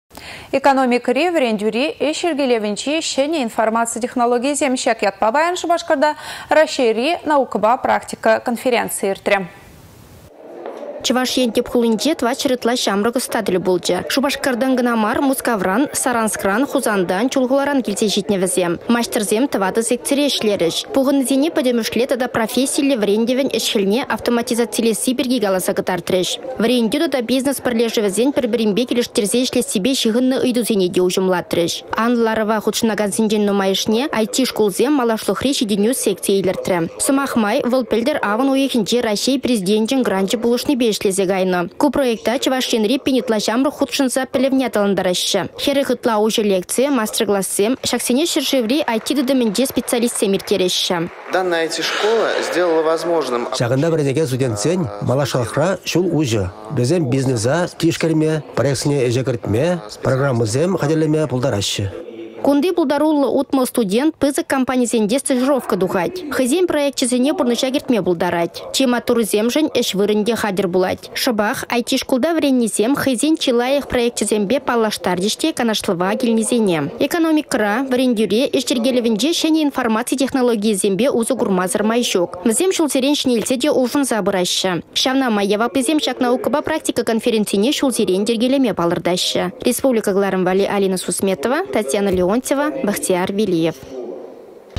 Конференцире — çĕнĕлĕхсем пирки
Экономикăри, вĕрентӳри, ĕç йĕркелĕвĕнчи çĕнĕ информаци технологийĕсем. Çак ятпа паян Шупашкарта Раççейри наукăпа практика конференцийĕ иртрĕ.